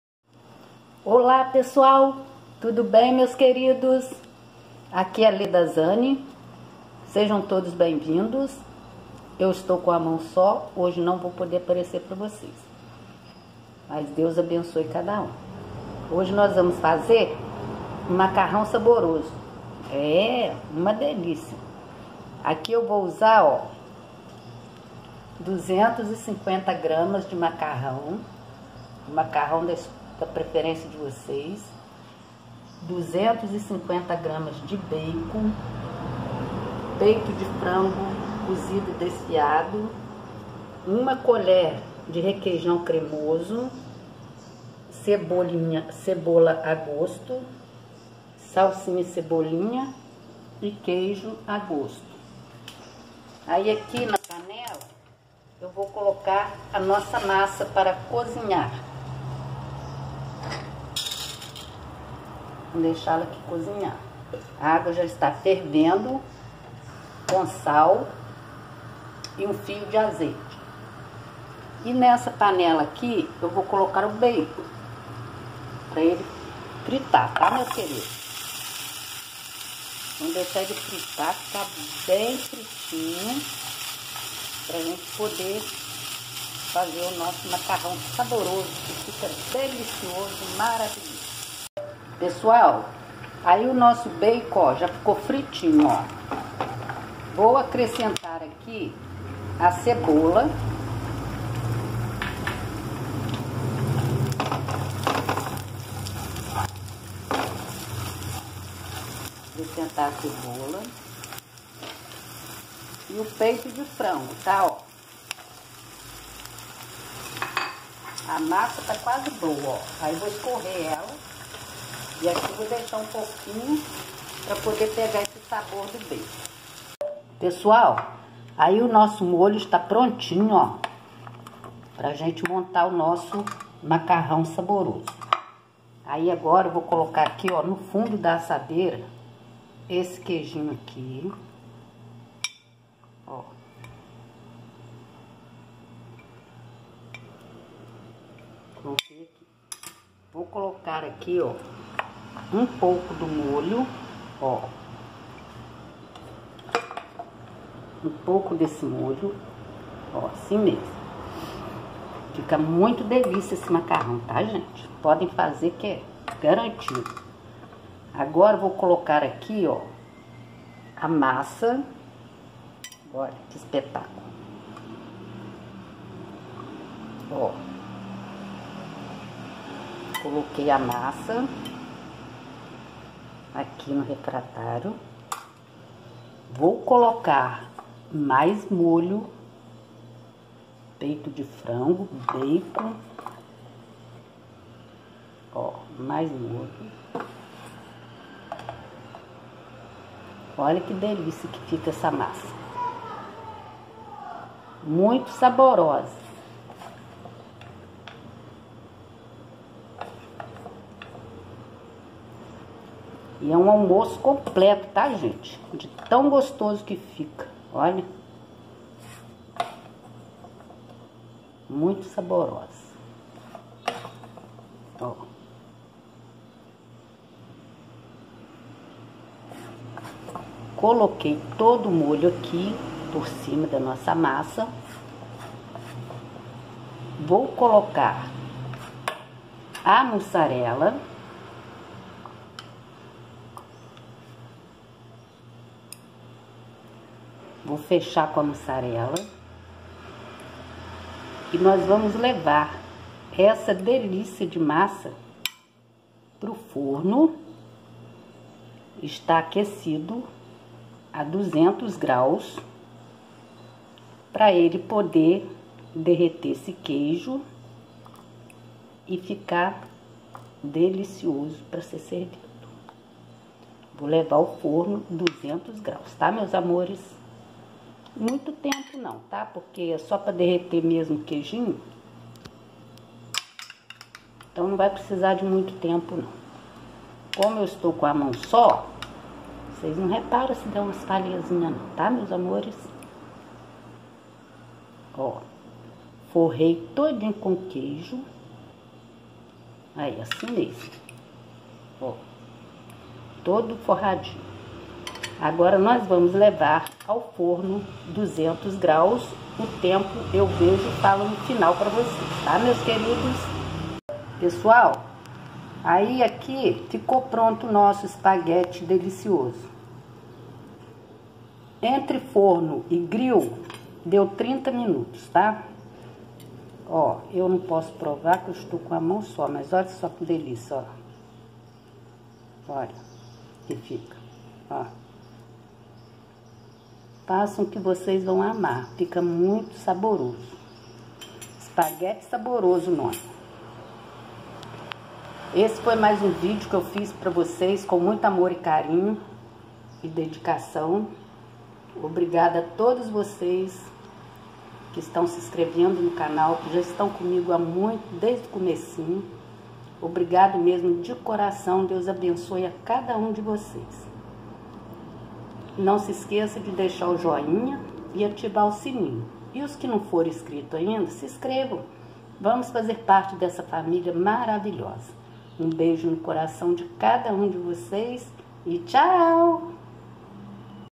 DELICIOSO SPAGHETTI AO FORNO!
Ingredientes
250 gr de macarrão
250 gr de bacon
250 gr de peito frango
Muzarela a gosto
01 colher de sopa de requeijão cremoso
Cheiro Verde a gosto
03 colheres de sopa de cebola